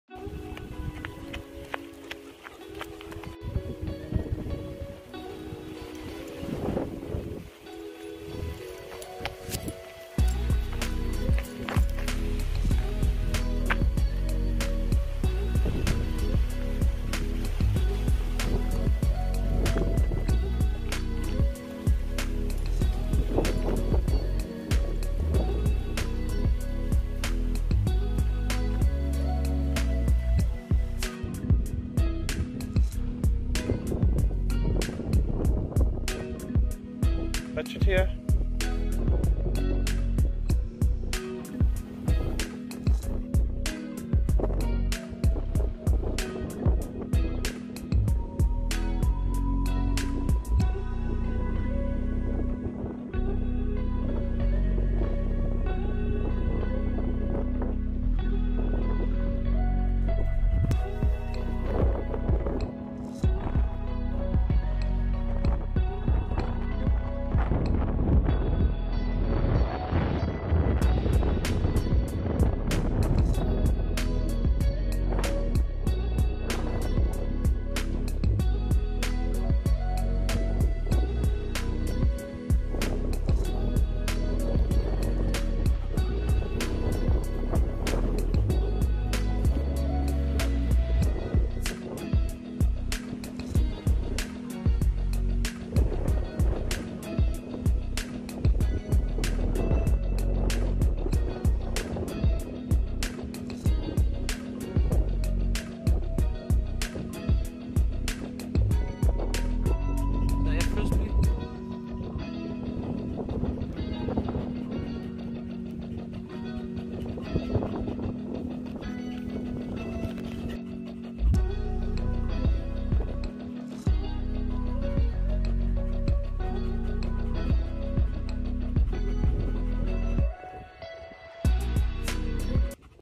A LEONBERGER AND A FRISBEE
a day out with Montana and her favourite toy the Frisbee and loving life ❤